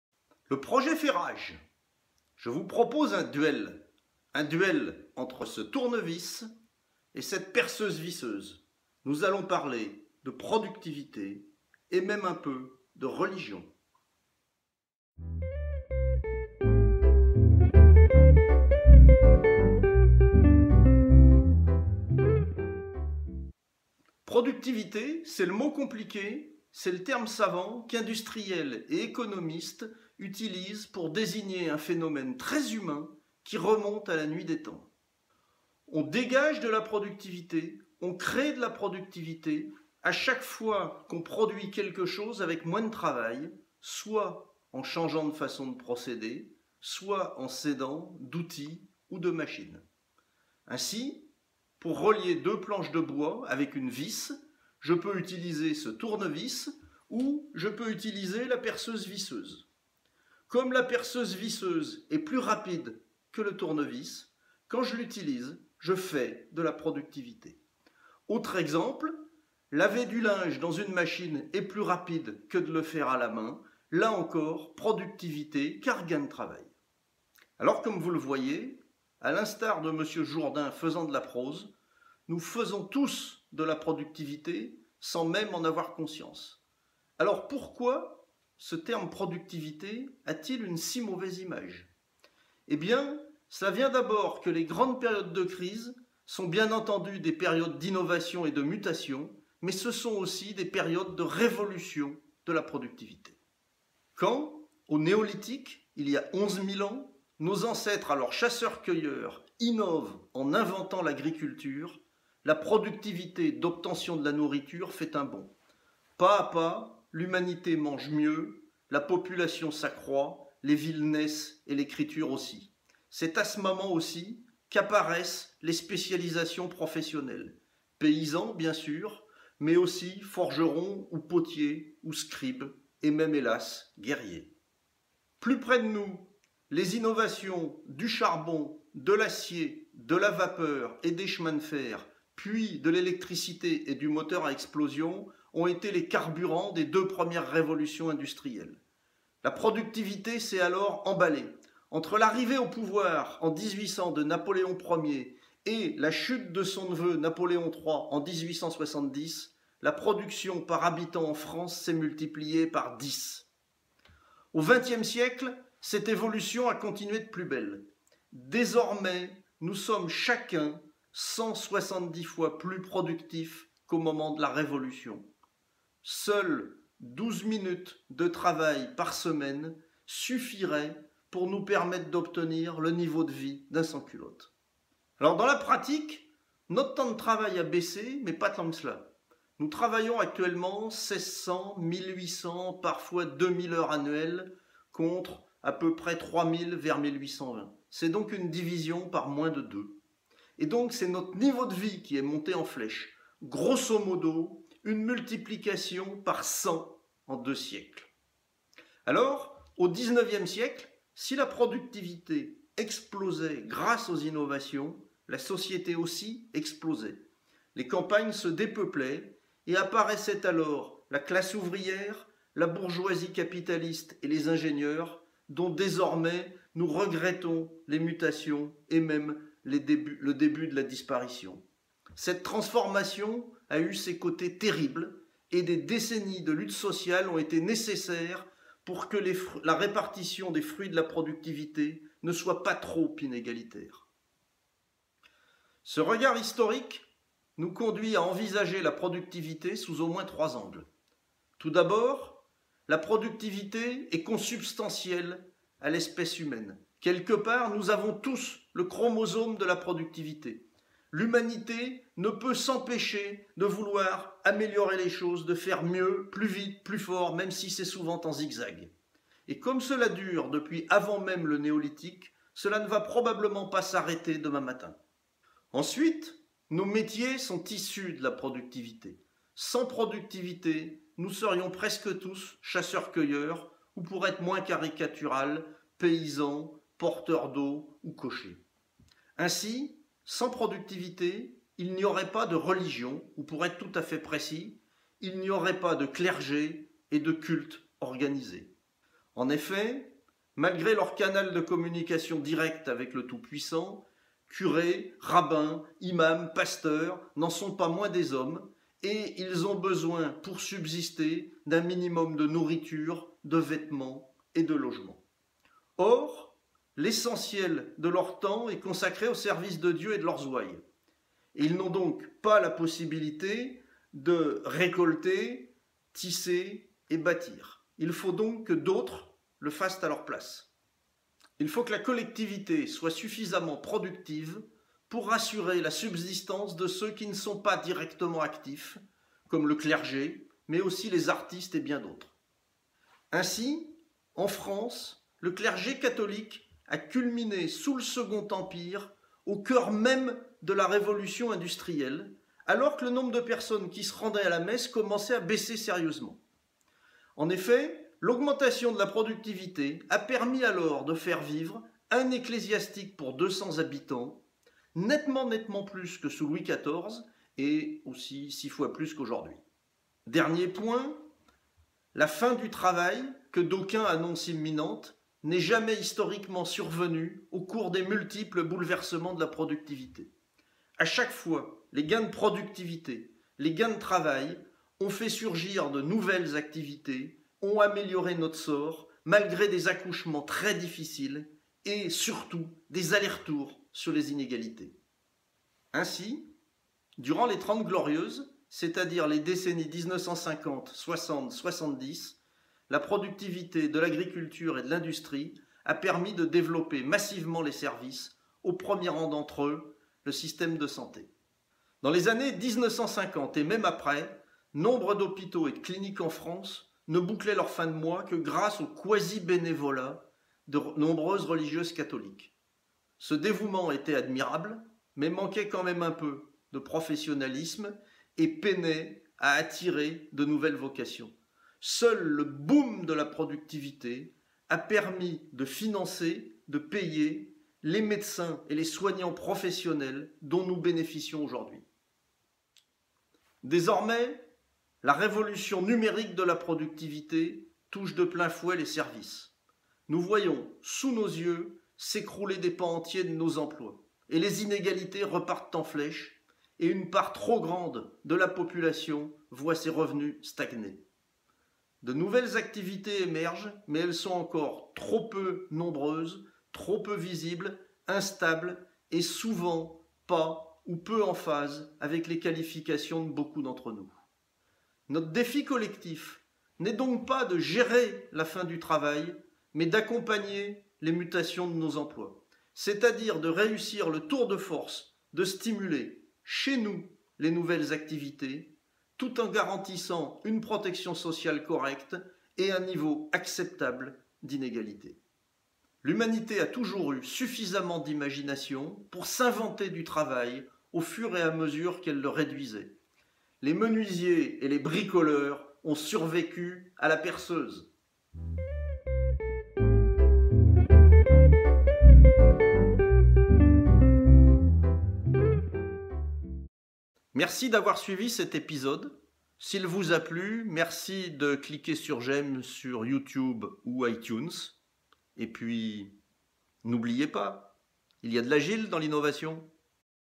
Pas de religions sans productivité LPFR#9
Qu’est-ce que la productivité ? Quels liens entre productivité et cultes ? En savoir plus

Mon livre "Développer un produit innovant avec les méthodes agiles" (Éditions Eyrolles)

Ces vidéos (et d'autres chroniques) sur le blog Humeurs Mondialisées